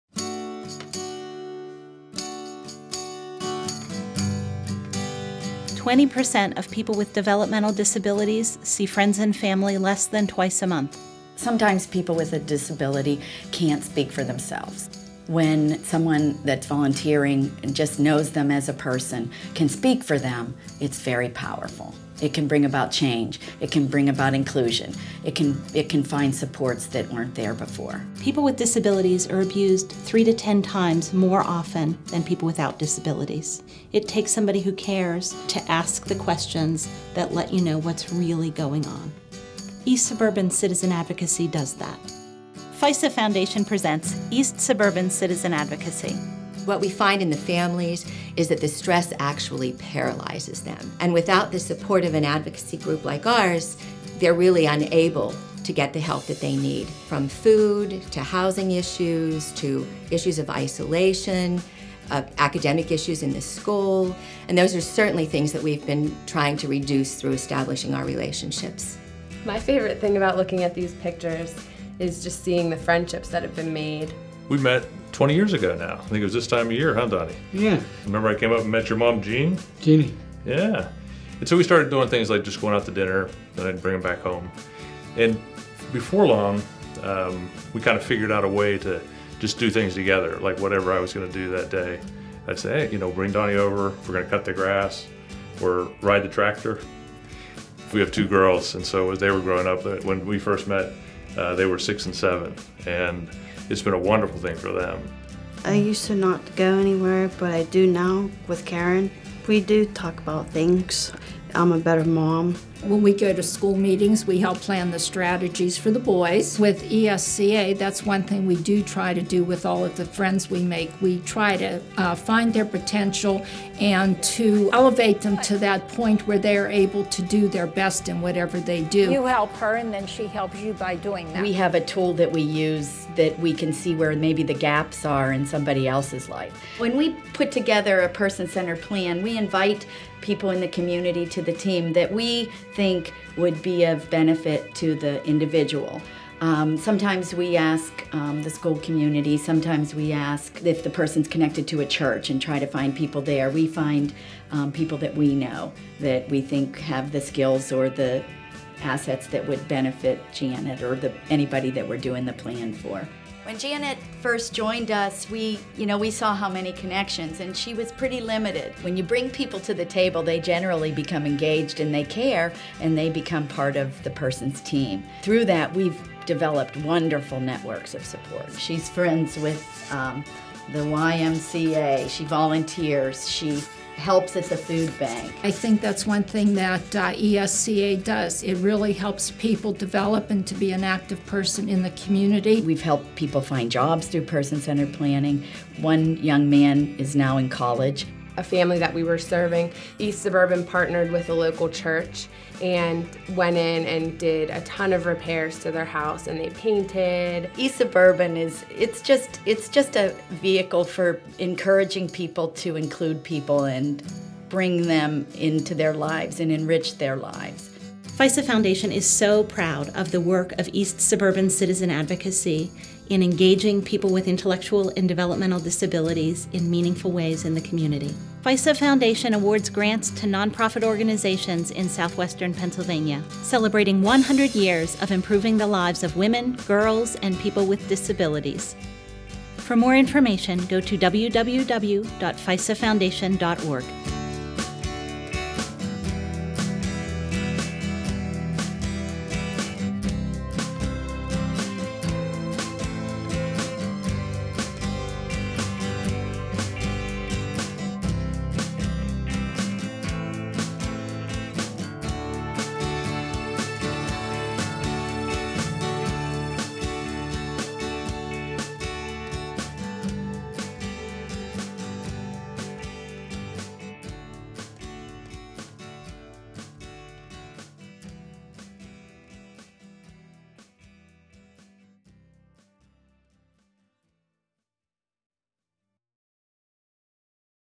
East Suburban Citizen Advocacy
In honor of FISA Foundation's centennial, we are highlighting the work of our grantee partners. Learn about East Suburban Citizen Advocacy.